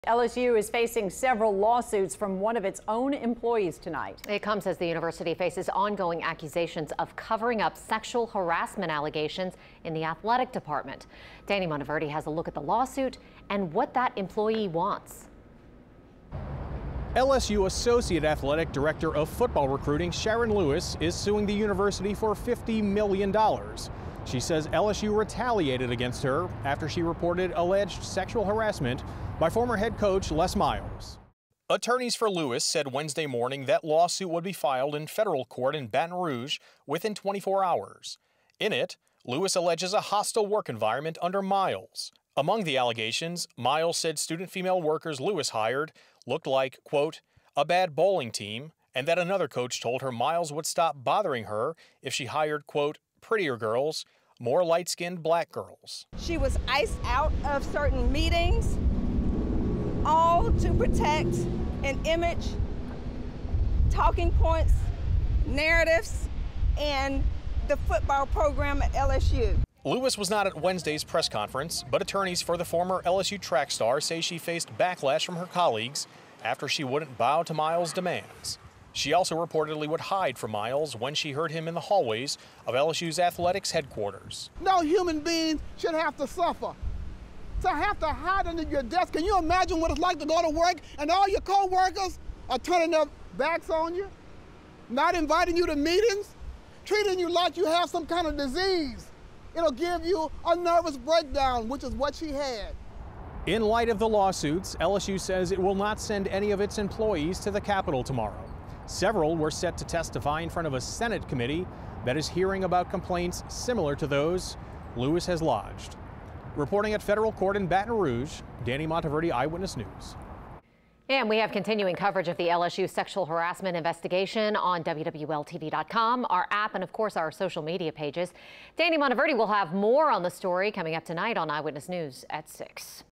Attorney: LSU acted as 'crime syndicate' in cover up of Les Miles' alleged sexual harassment
The attorneys plan to hold a press conference at 11 a.m. Wednesday at the 19th Judicial District Court in Baton Rouge.